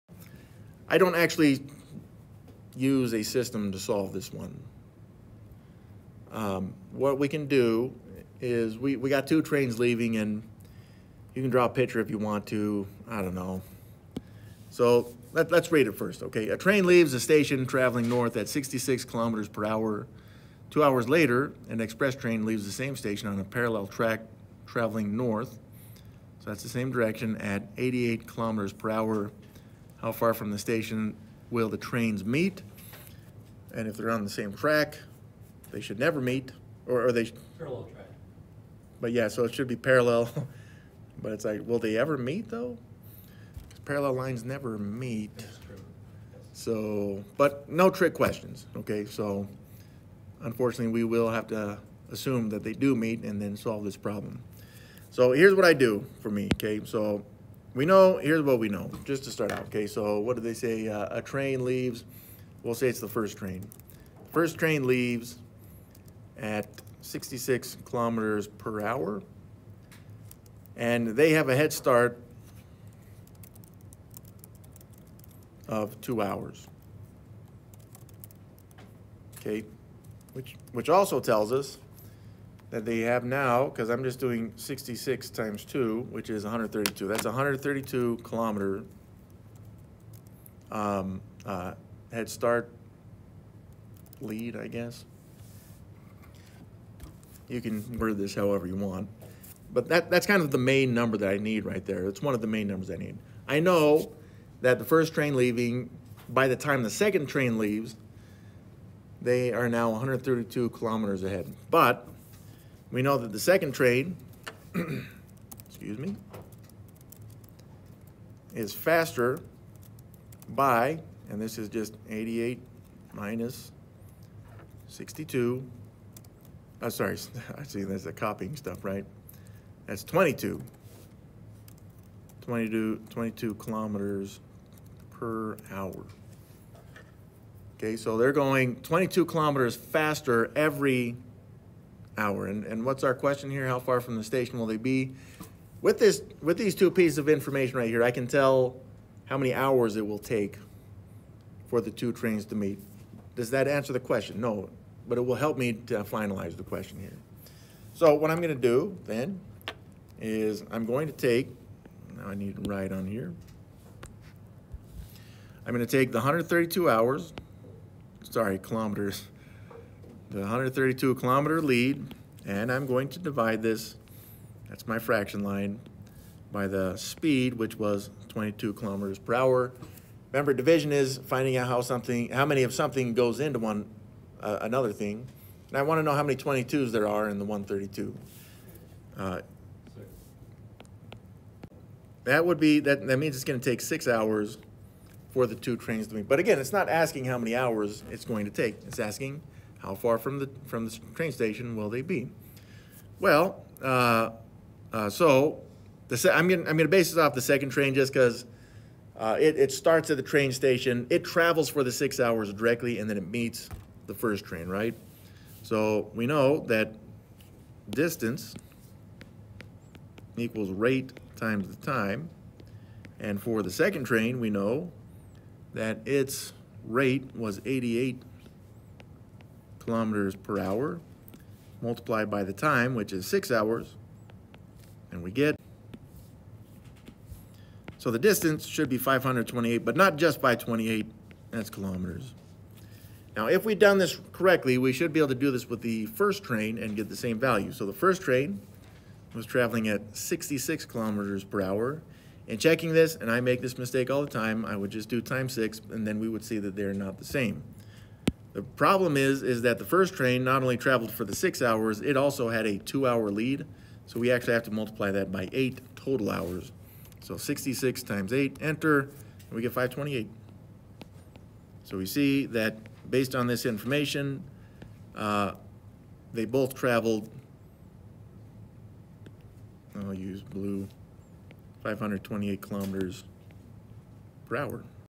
2 Variable Systems of Equations Applications: the Trains Meeting Example
A train leaves a station traveling north at 66 km/h. 2 hours later, an express train leaves the same station on a parallel track traveling north at 88 km/h.
How far from the station will the trains meet?